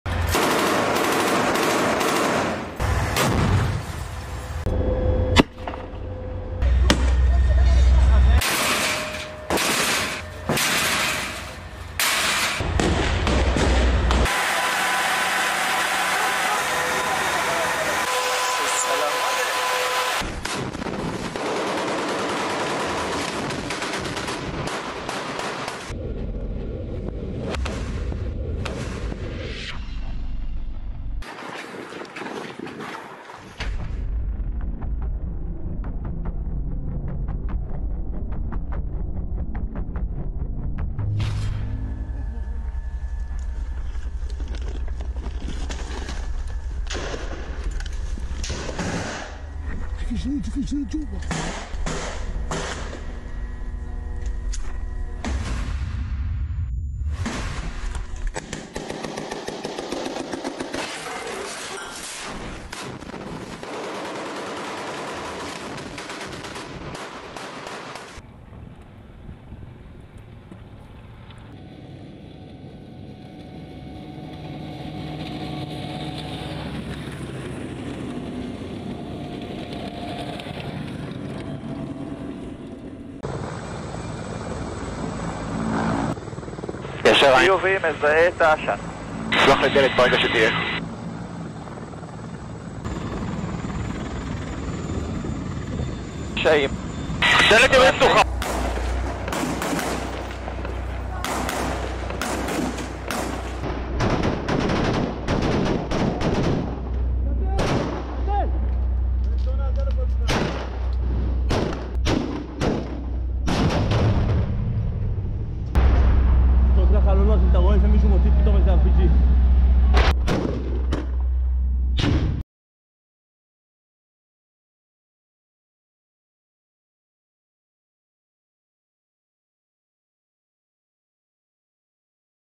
Israeli forces target Hamas terrorists with heavy machine gun fire in Gaza
Israel’s Nahal Brigades battled Hamas in the north of the Gaza Strip after spies picked up their presence in the Jabalia area. The 931st Battalion led the attack, followed by the 551st division. Hamas and Islamic Jihad fighters used RPGs to ambush the Israelis and hidden mortar positions to fire back against their Merkava tanks.